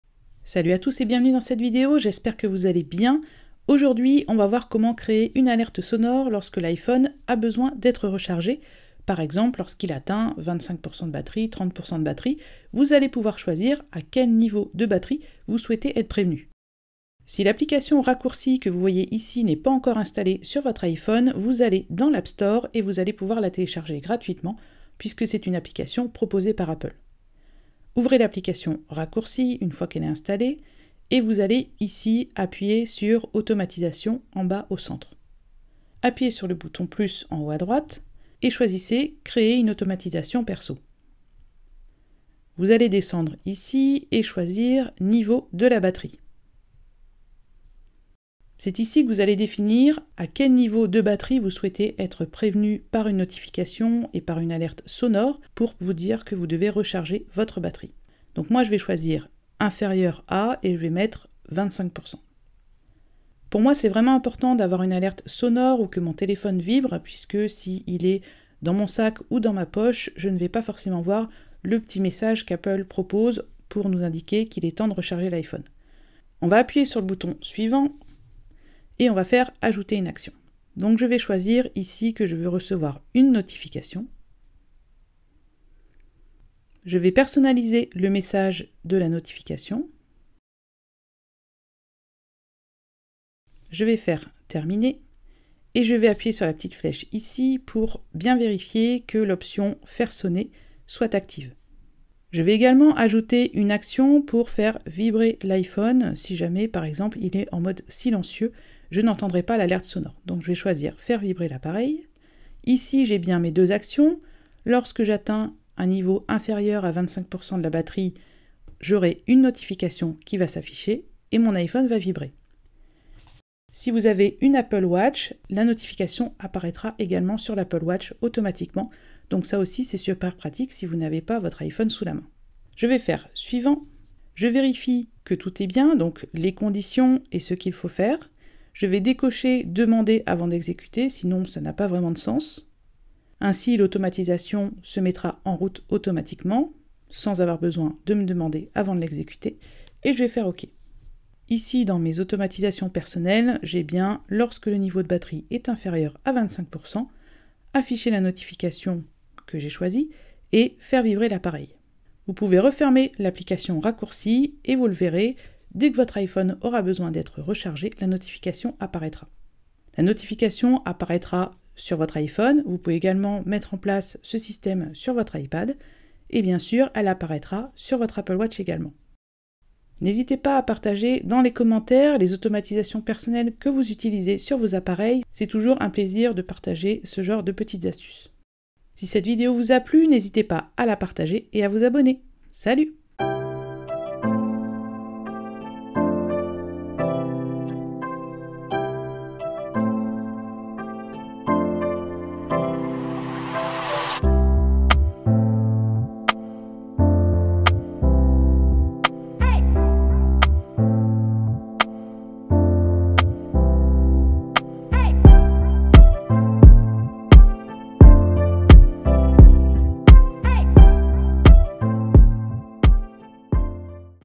Créer une alerte sonore en cas de batterie faible sur iPhone et iPad ! 🤩🔋
⇢ Dans cette vidéo, je vous explique comment créer une notification avec alerte sonore et/ou vibration lorsque votre iPhone n'a plus de batterie !
🔗 Liens de la vidéo :
• iPhone 12 mini
• AirPods 3
🔥 Vous avez aimé cette vidéo ? Montrez-moi votre soutien grâce au bouton "Merci" 😌